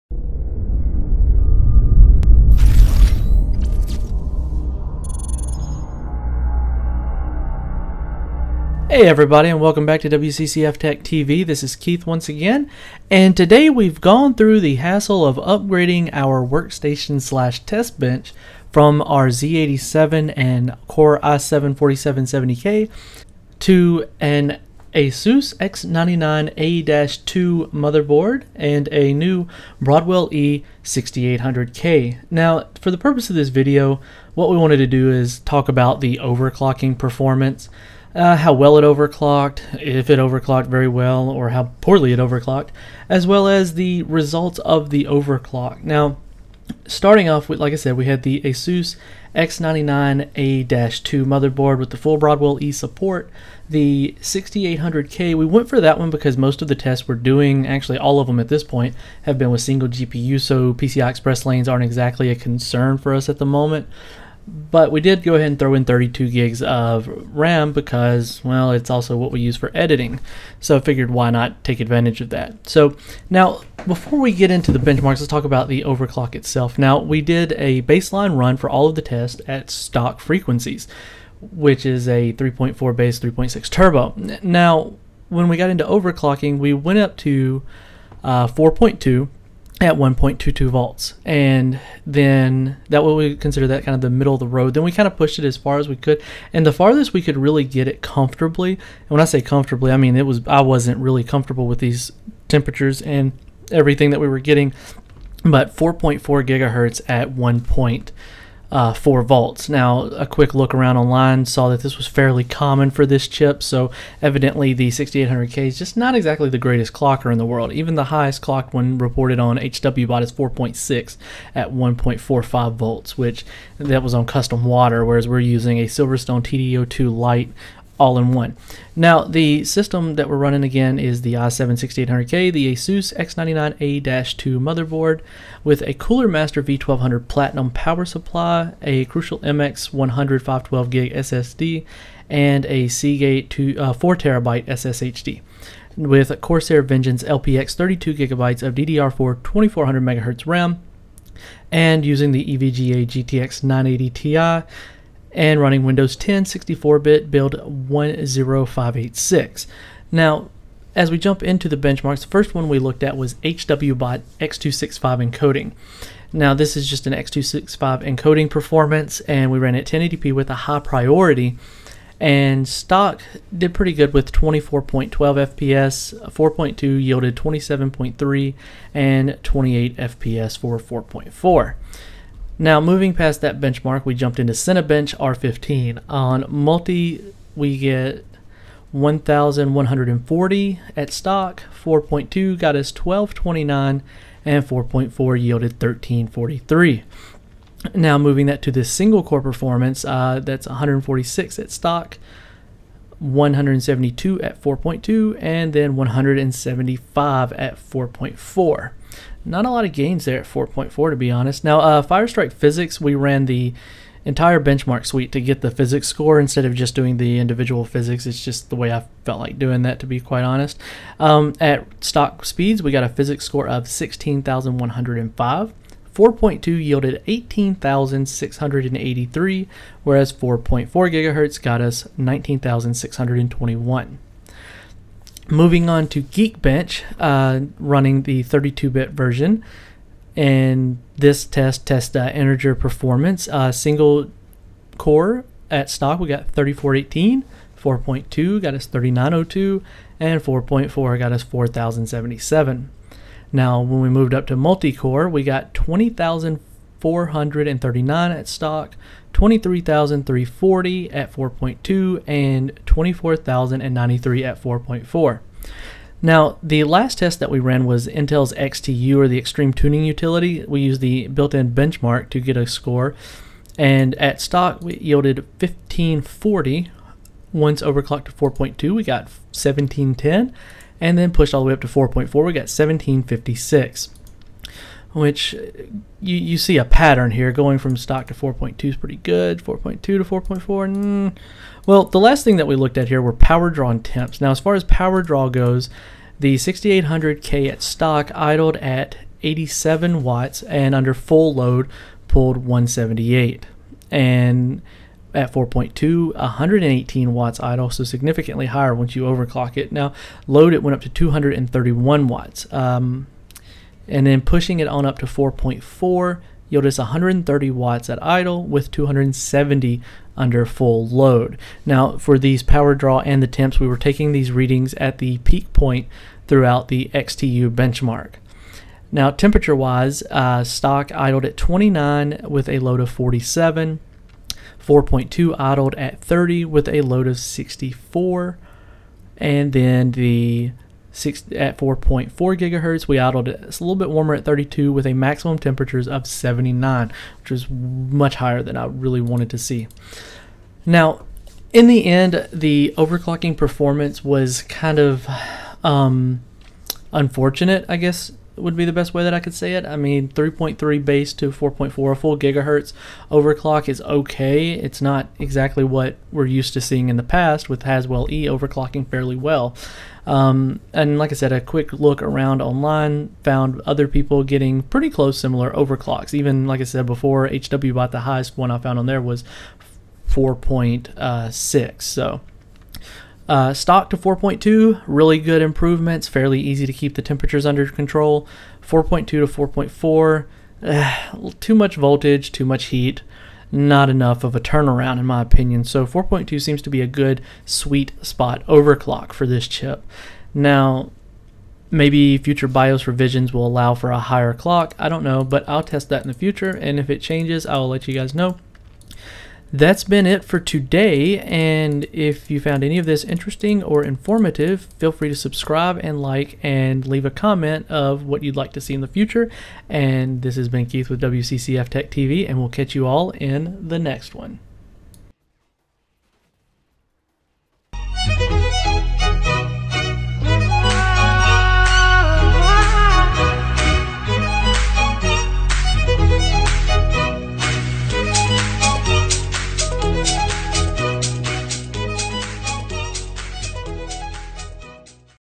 i7 6800k Overclocking Performance Review
We take our i7 6800k to the ringer and see how much performance gains we could squeeze out of it.